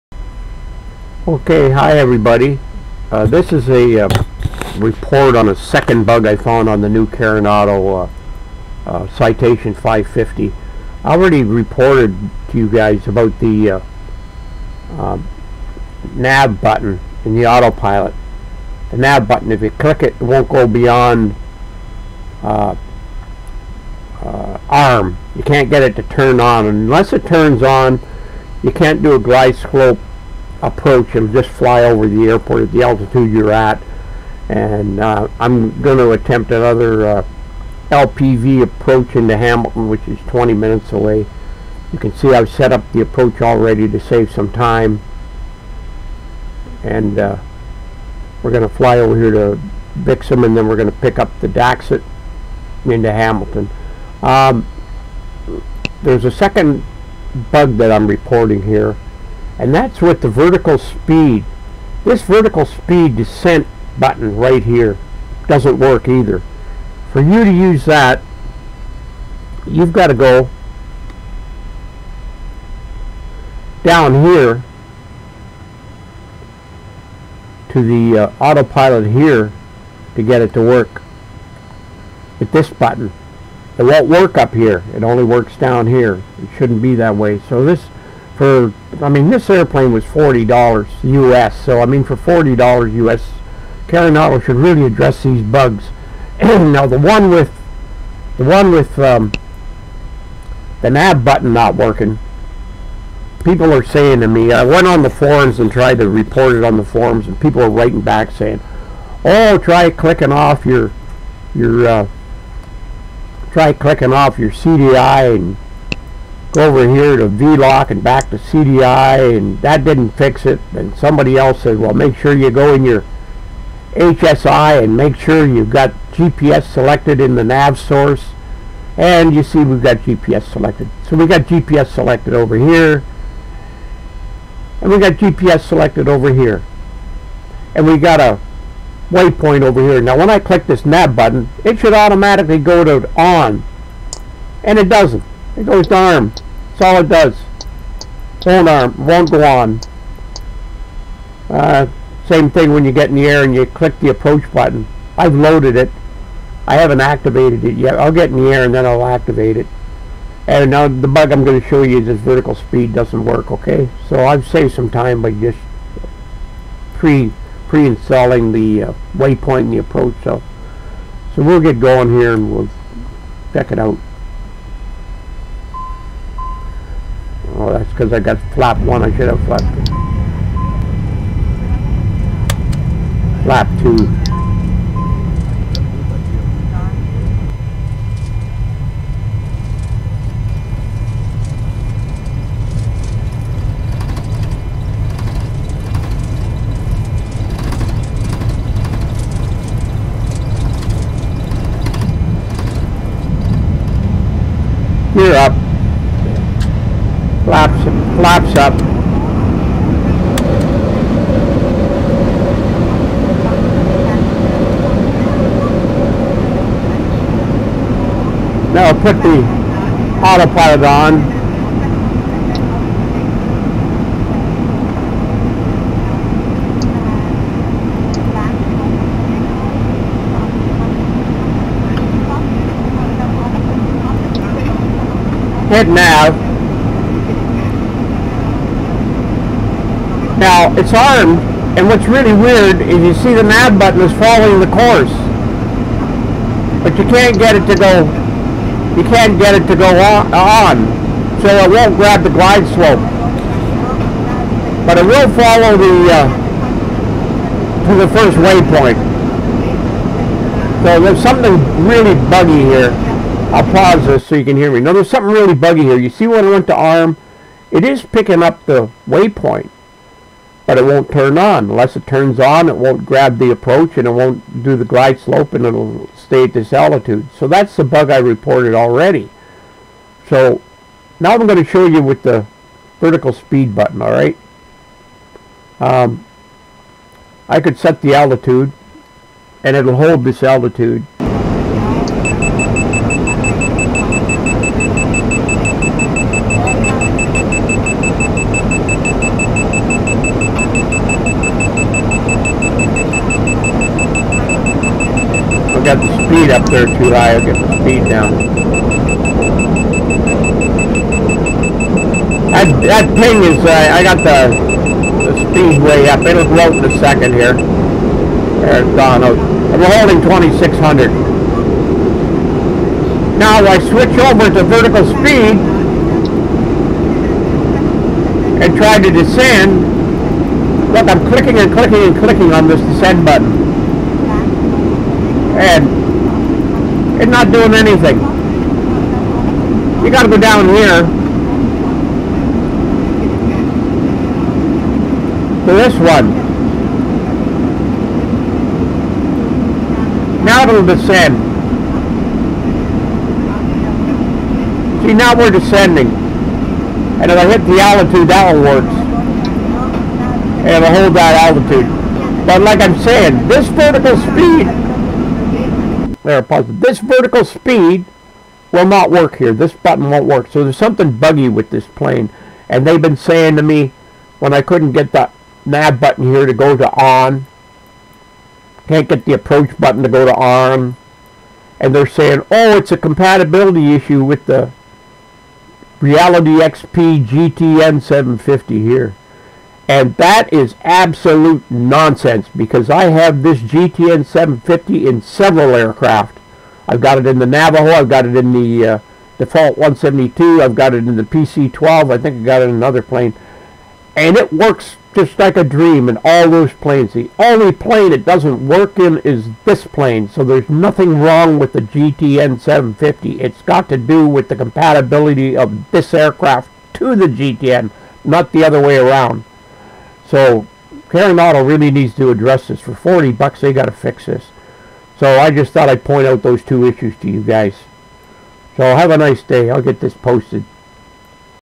Another Bug in the Carenado Citation 550???
Found a couple bugs in the new Carenado Citation 550, here's another bug I found. Even did a fresh re-install, didn't help